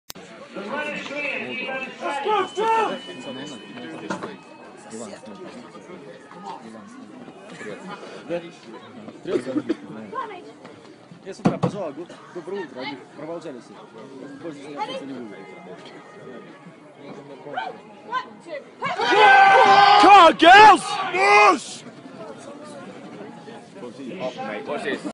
Push start in St. Moritz
Mica McNeill and Rebekah Wilson.
Season 2013/2014. European Cup.